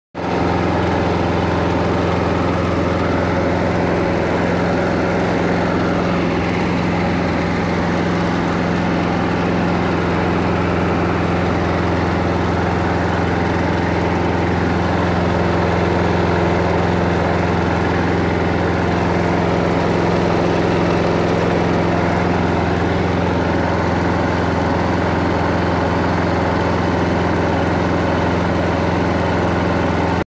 Atco Admiral 16 Rotary Lawnmower
Atco Admiral 16 Lawnmower running after service. This was a none runner prior to servicing.